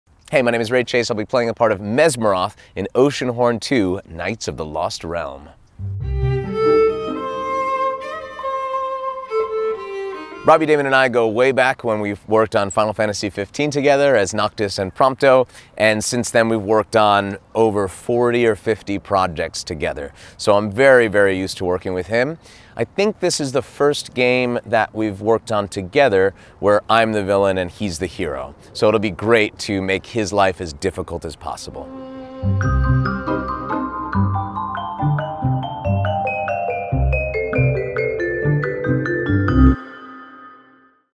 Oceanhorn 2: Ray Chase (Mesmeroth) vs Robbie Daymond (Hero)
Final clip from our Ray Chase interview before we drop the full video. Ray reveals that this is the first time he plays a villain, and Robbie Daymond (you heard him in Final Fantasy XV, Persona 5, Fire Emblem, and many other games) the hero. The two have worked together in more than 40 projects.